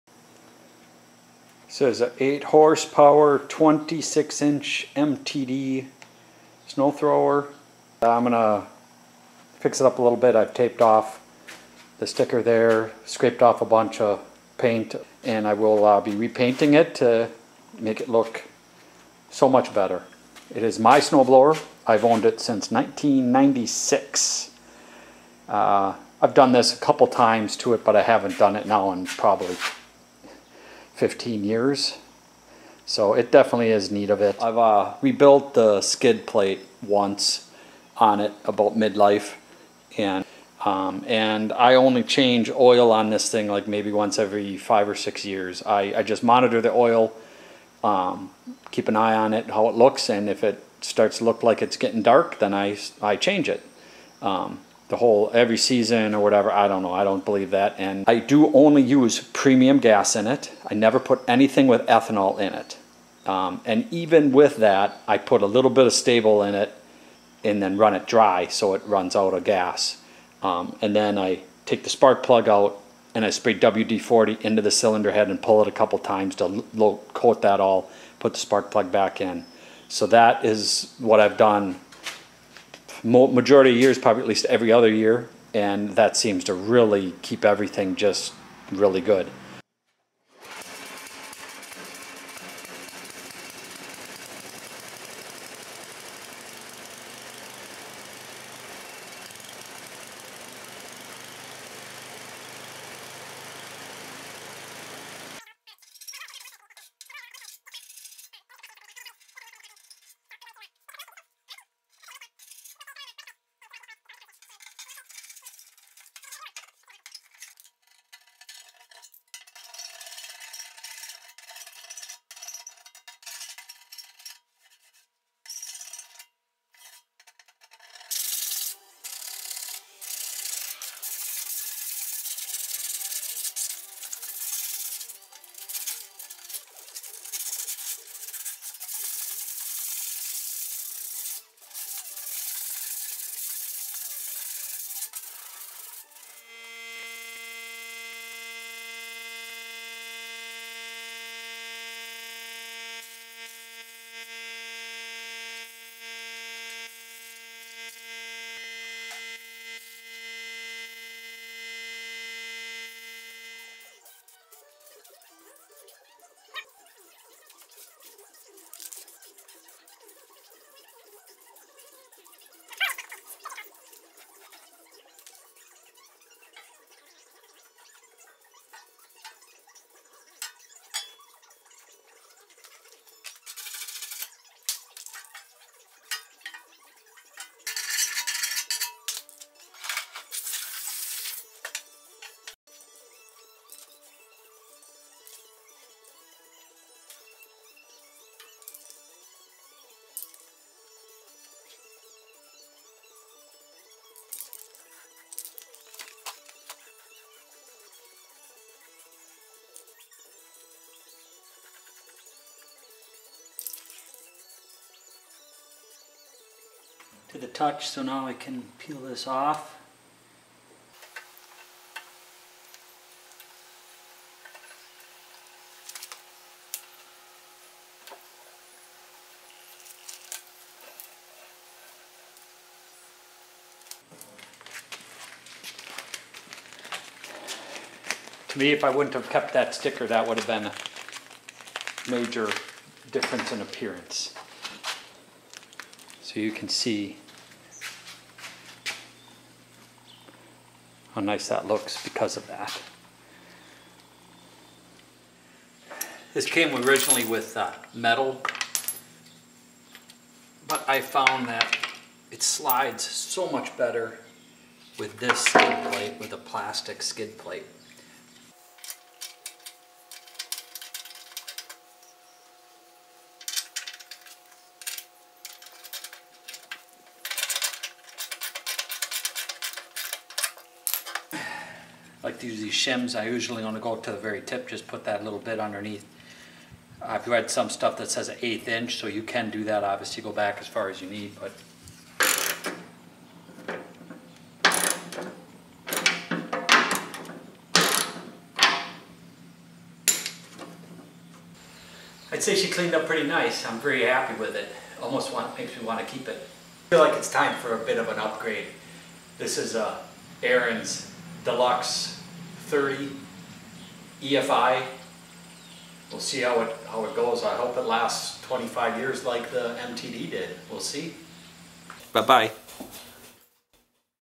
Make an old snowblower look new, well sort of...
Painting my 1995 MTD.  Down and dirty!